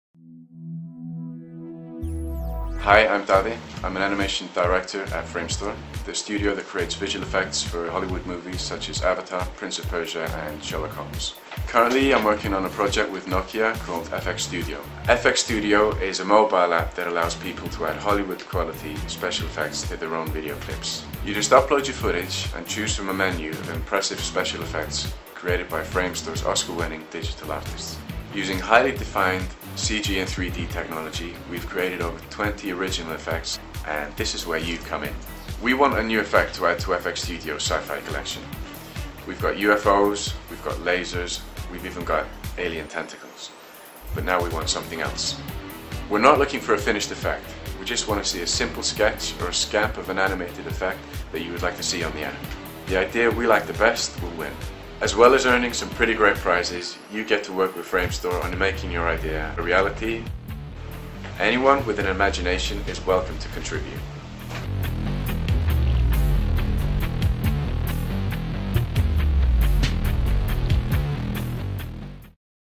Nokia N8 FX Studio1 (video 1)
Il Nokia N8 continua a far parlare di sé, alimentando l'interesse non solo di noi utenti appassionati di

smartphone, ma anche di aziende di sviluppo software. Una di queste è la Framestore, specializzata nella produzione

di software per gli effetti speciali e per la post produzione video.

Insieme a Nokia, la Framestore sta lavorando a un nuovo progetto che ha come obiettivo lo sviluppo di un

applicazione mai vista finora per i dispositivi mobili! Si tratta di FX Studio Phone, il software in grado di

aggiungere straordinari effetti speciali 3D nei video realizzati con la fotocamera da 12 Megapixel del Nokia N8.

Info e video presi dalla rete.

(Vuoi ricevere notizie hi-tech? Registrati alla mailing list di soluzioni informatiche all'indirizzo